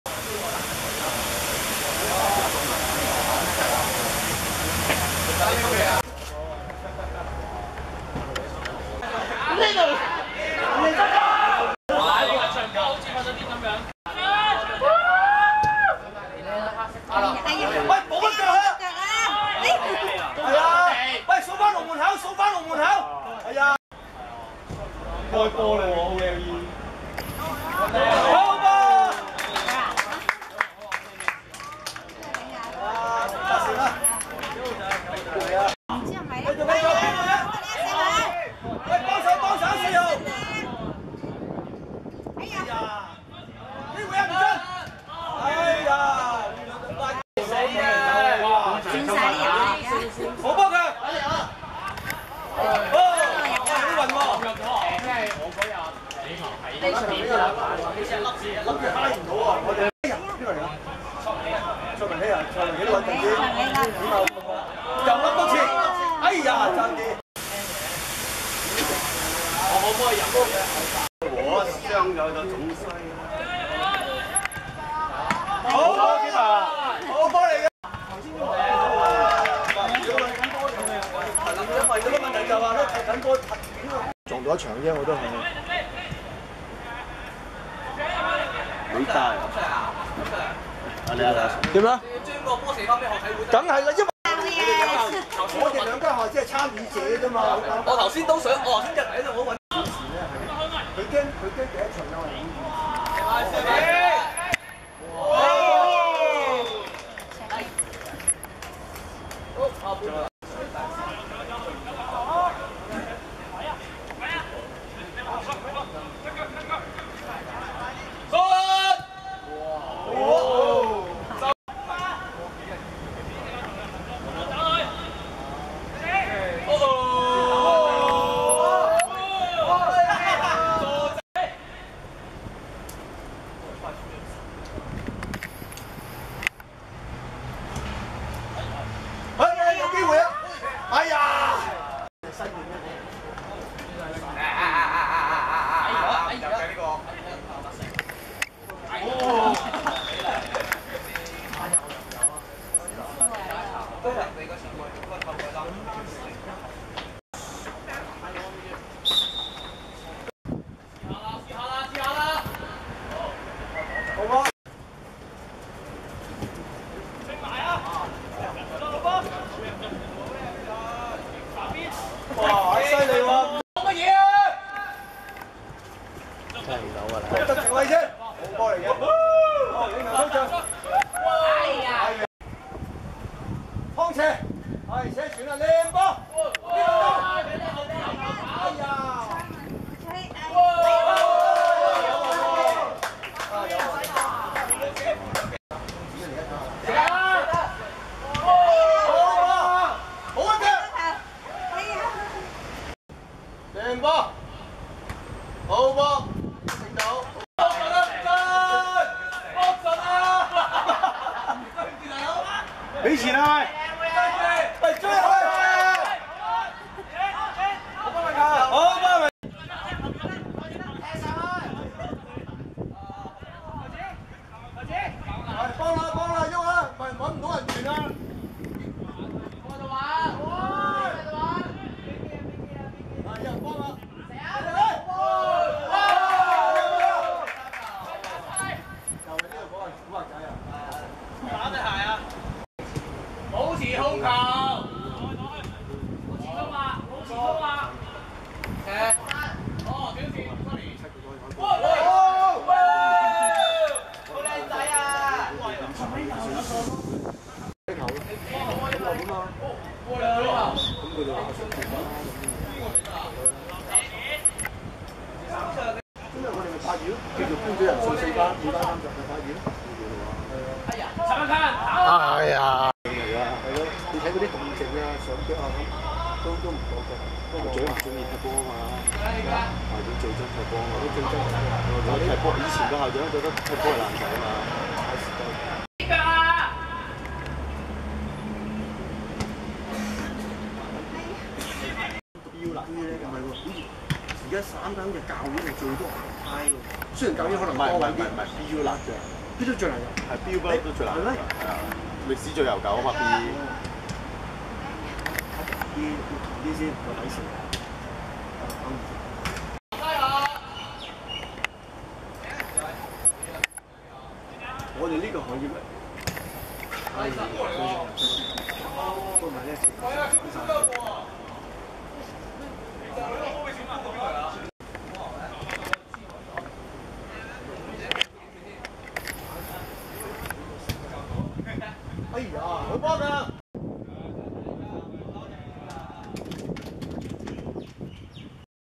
翁祐vs張煊昌(2015.9.24.元朗學界足球甲組)精華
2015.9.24.天業路仿真草塲 14:30 翁祐(黃) 0:11 張煊昌(紅)